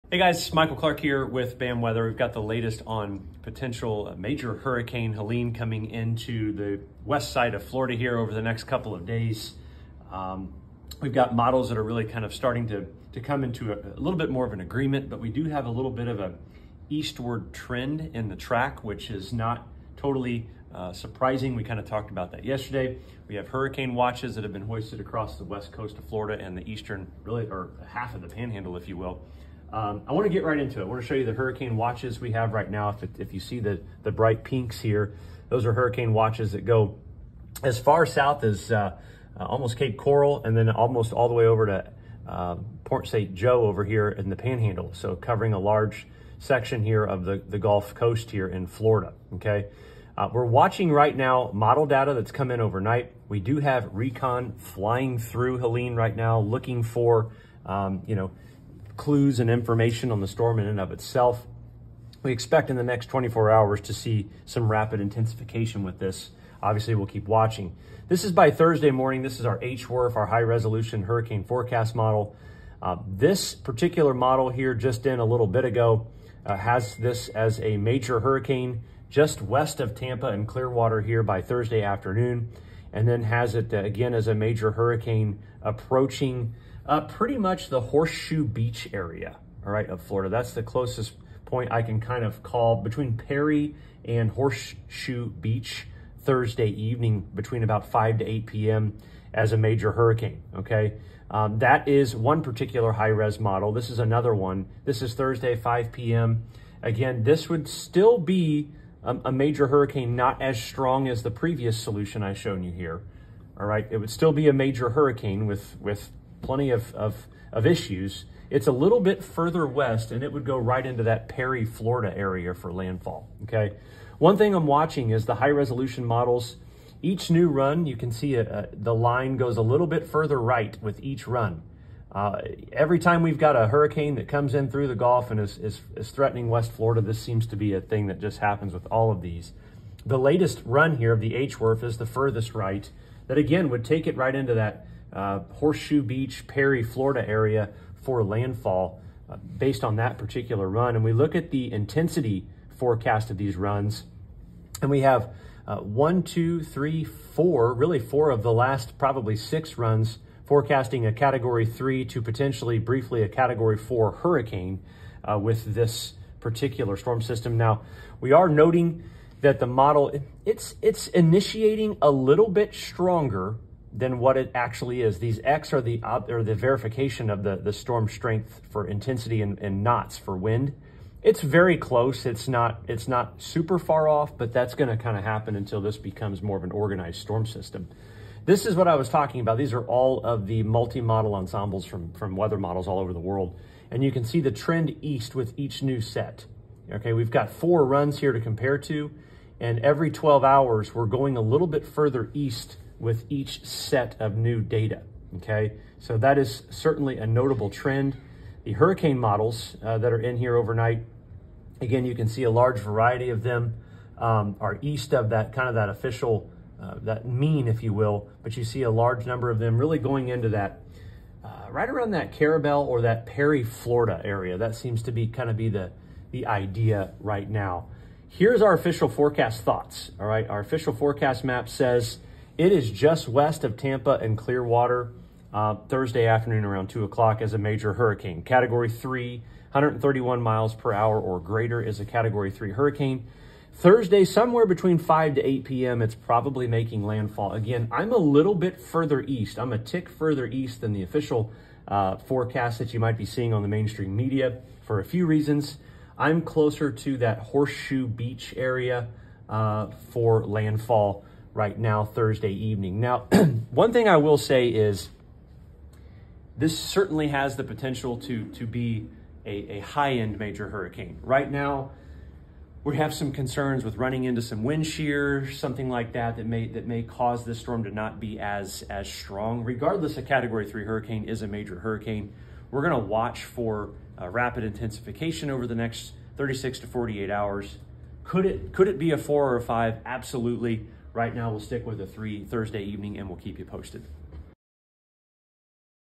Eastward Shift in Likely Major Hurricane Helene: Here's a Meteorologist's Update
🌀An eastward trend in hurricane models last night, creates an eastward shift in landfall for Hurricane Helene.

As of now, favoring landfall near Horseshoe Bend, Florida as a category 3 hurricane Thursday evening.

Areas south of landfall (Tampa, Ft. Myers, Sarasota) will also be feeling a great deal of impacts from Helene. Storm surge of 5-8 ft, tropical storm force winds, and heavy rainfall.

📲We will continue to keep you updated, so be sure to turn on your notifications or sign-up for Clarity for all of our updates sent straight to your device